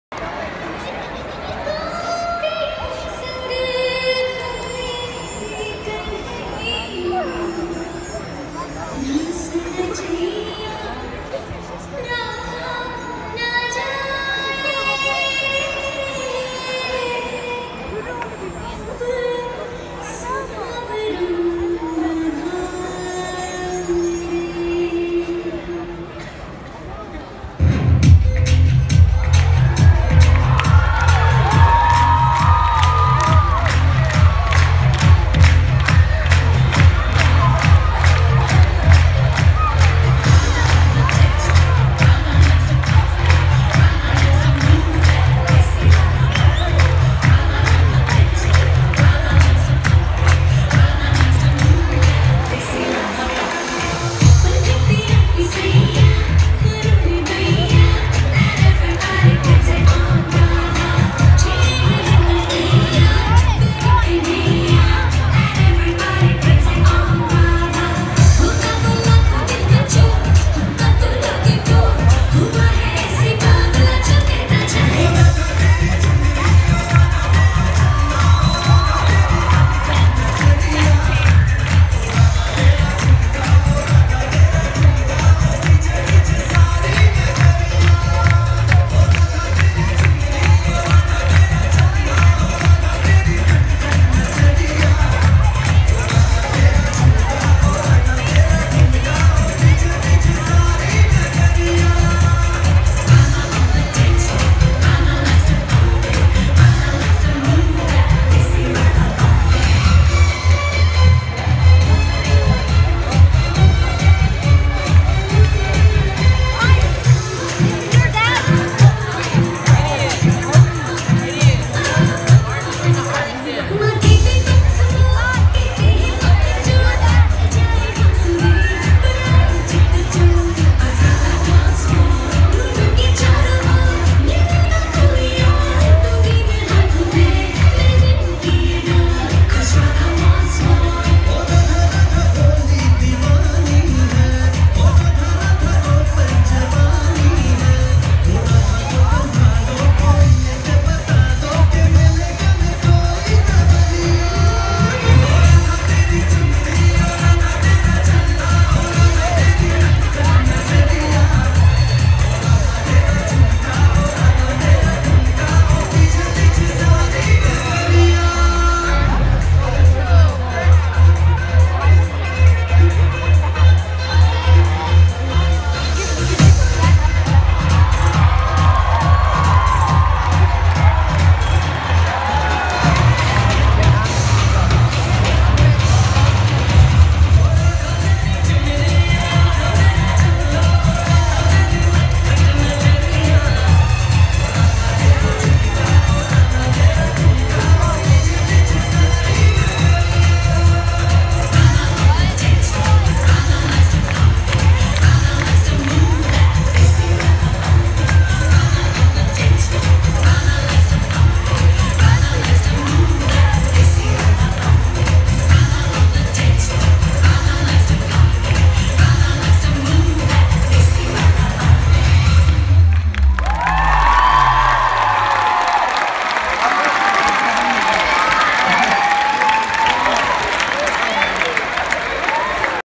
Bollywood opening dance
She had trouble hearing in the gym with the noise and also since her aids need fixing. Yet when it comes to dancing we never give up. She performed one of her fav no as the opening performance at school today for international day! It was overwhelming for me to hear the applause a no if times during her dance! As always my hands shook due to emotions. Dream coming true that she would dance and enjoy music! So grateful to the school for giving us an opportunity and believing in us! To be loved and applauded was so heart warming! My heart sang as I saw her being appreciated, hi-fived, clapped for after the performance wherever she went! Thank you Alaka ma’am for this day and every day! Who says deaf can’t dance!